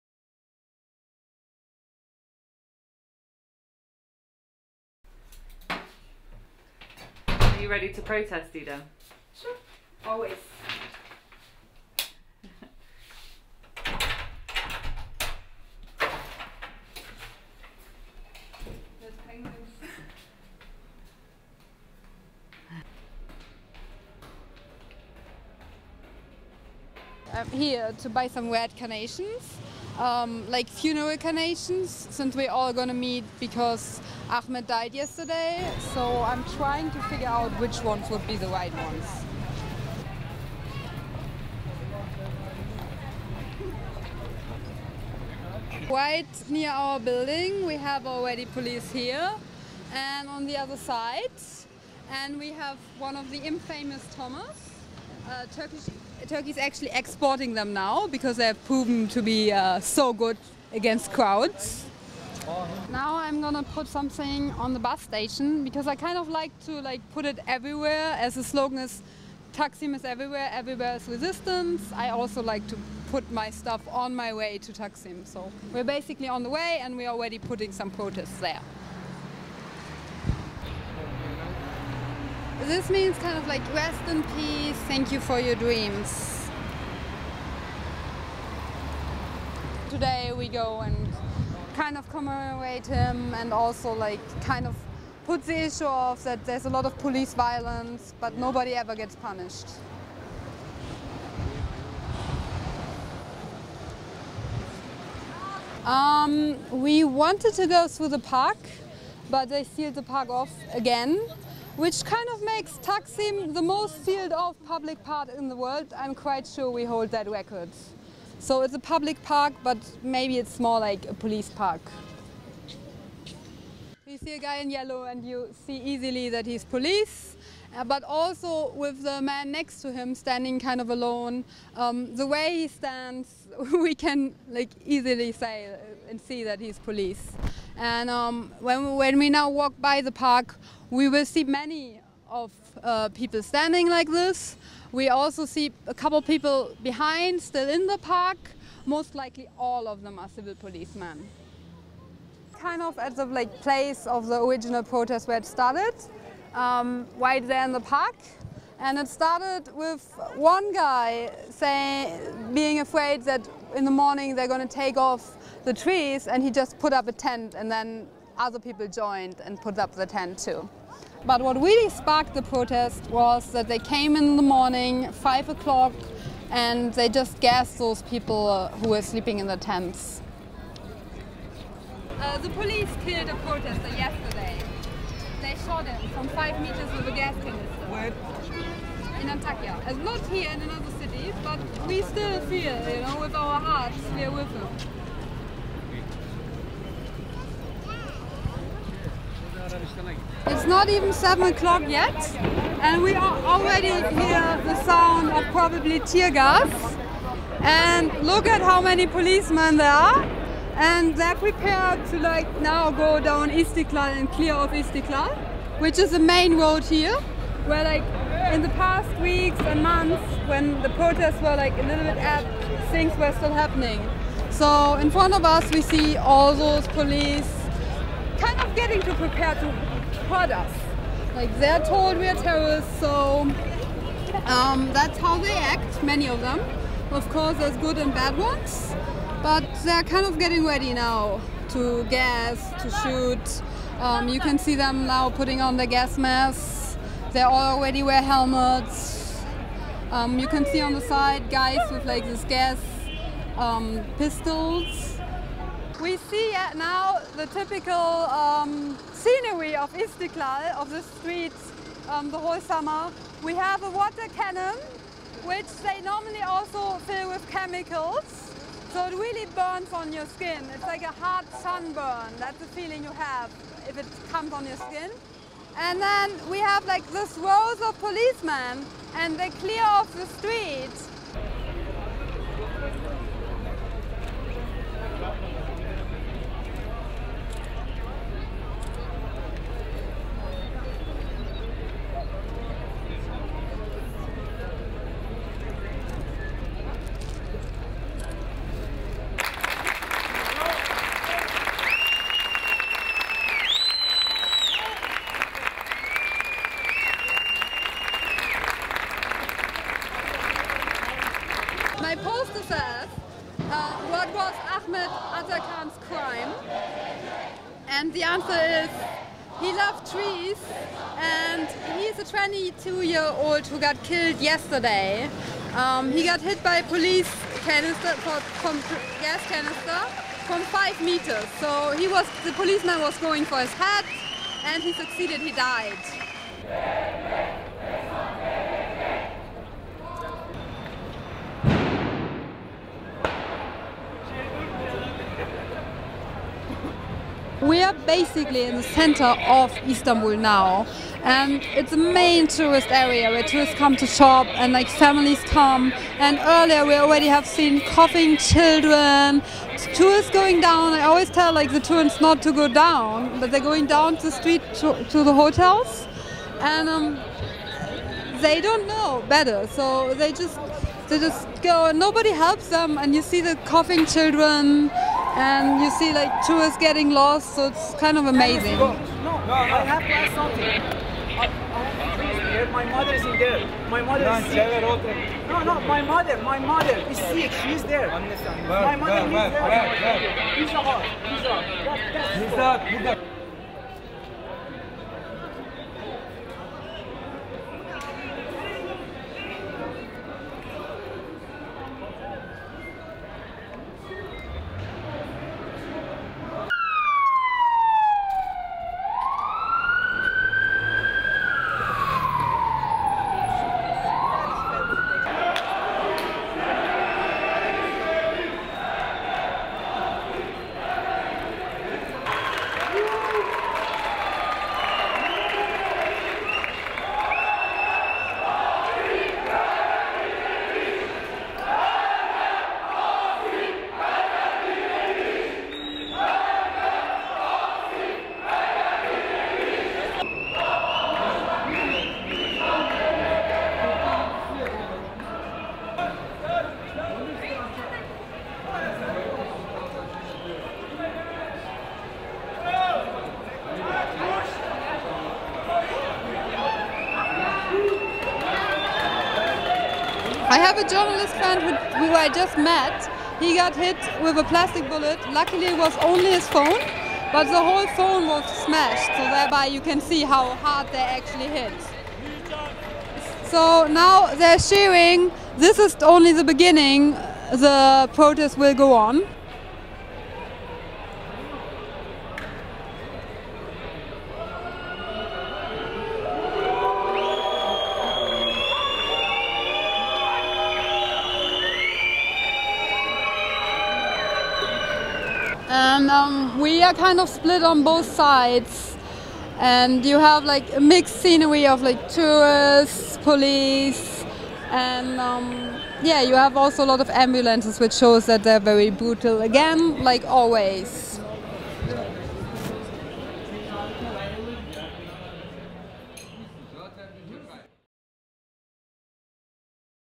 RIP AhmetAtakan - typical gece capulling Taksim'de, Summer 2013
Police now use plastic bullets openly on anything that moves - so if you find yourself caught up in Taksim, don't run, WALK. Didem an original Gezi Park protester shows me around her world. The police staged a fresh crackdown on demonstrators in the early evening hours of Sept. 10, after groups had gathered in Istanbul's Taksim Square to denounce the death of a protester in Antakya. The Taksim Solidarity Movement, now outlawed, called for a vigil to remember the six protesters who have died at the hands of the police. Later a young man was detained for holding Didem's poster. We don't know what happened to him.